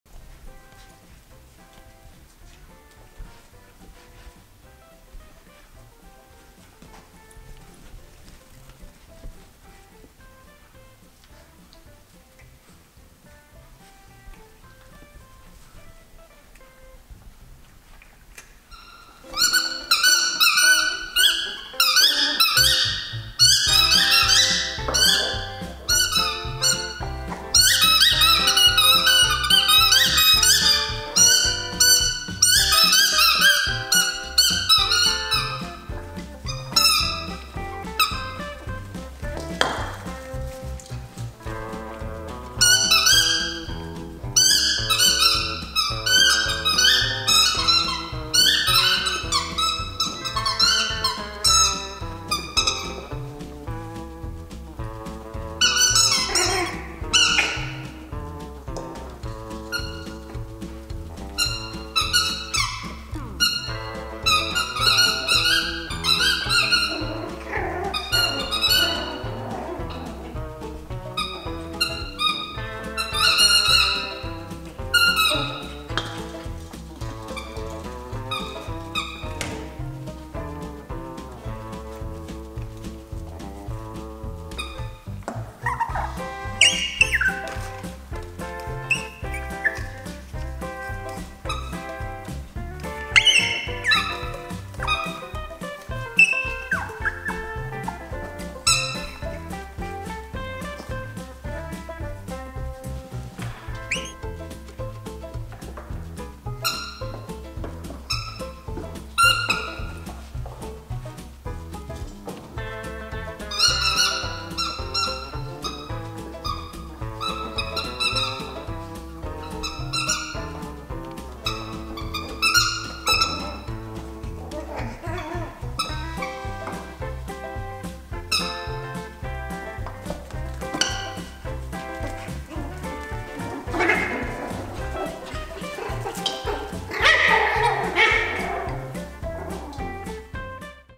Mini Poodle Puppies (8 weeks old) | Playtime and Exercise!
Here's a short video of our four adorable Mini Poodle puppies running around the play area and burning up some energy before their afternoon nap time! Lottie is the cream colored female, Pixie is the small red colored female, Monty is the apricot colored male, and Onyx is the black male puppy.